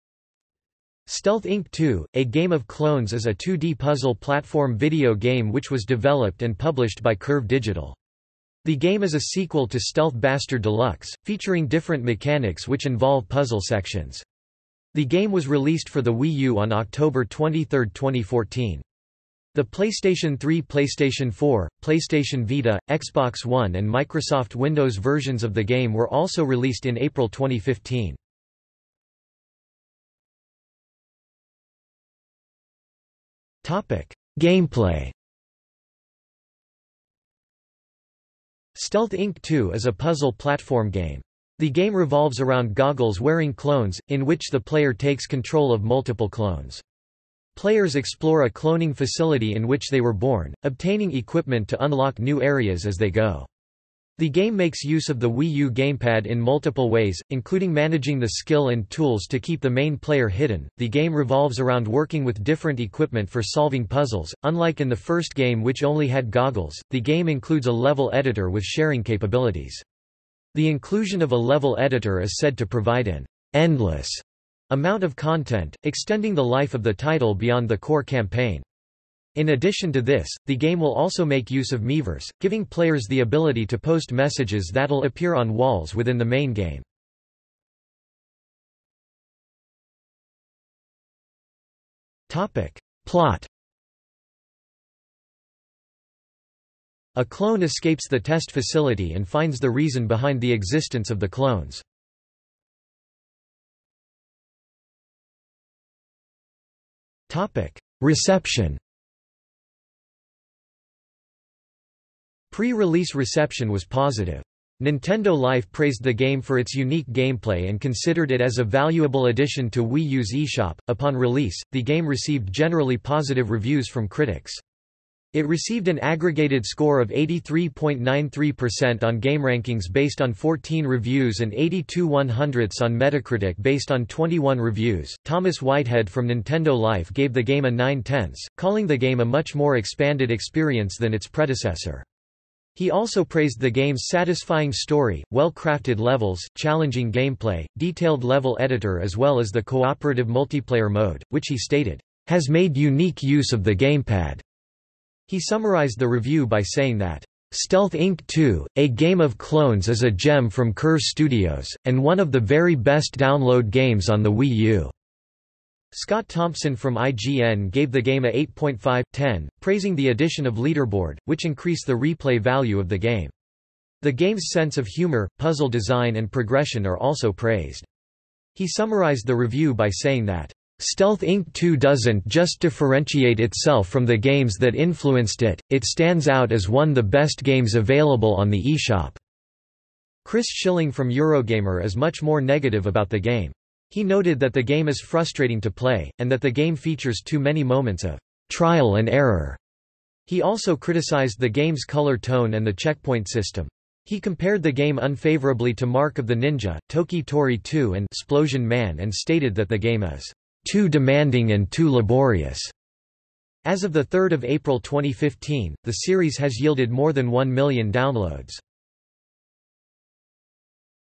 Stealth Inc 2: A Game of Clones | Wikipedia audio article
00:00:30 1 Gameplay
00:01:28 2 Plot
00:01:43 3 Reception

- Socrates

SUMMARY
Stealth Inc 2: A Game of Clones is a 2D puzzle platform video game which was developed and published by Curve Digital. The game is a sequel to Stealth Bastard Deluxe, featuring different mechanics which involve puzzle sections. The game was released for the Wii U on October 23 2014. The PlayStation 3, PlayStation 4, PlayStation Vita, Xbox One and Microsoft Windows versions of the game were also released in April 2015.